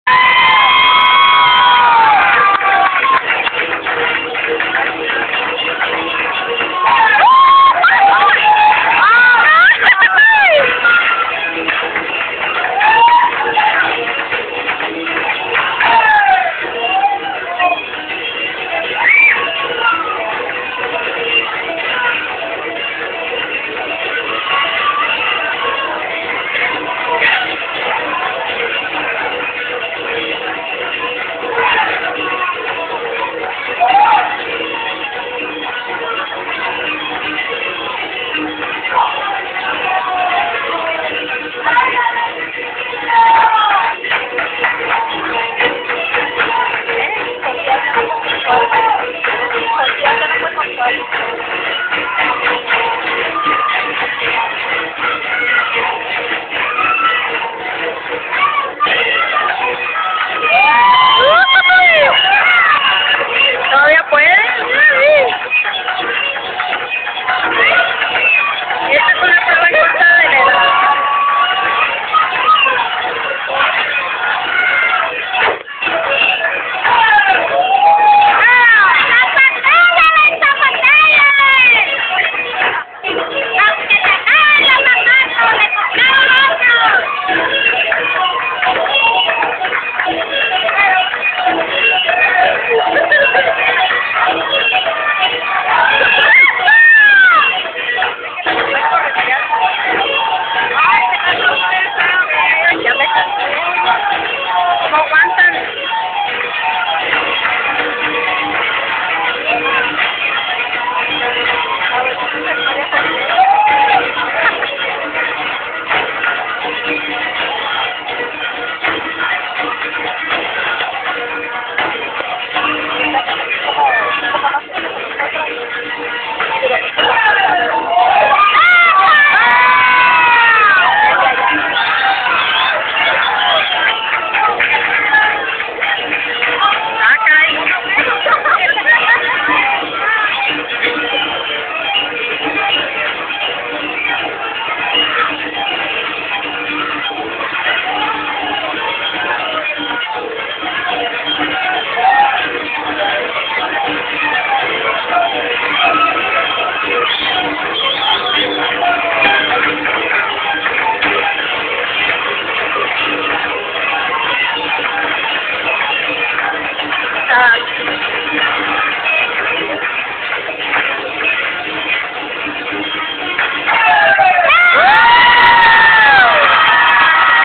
Folklorico de Exhibicion, Veracruz Presentacion Local
Presentacion Local. 16. de Septiembre.
P. Zamora, P. Franco, Josías Delgado (Resistencia)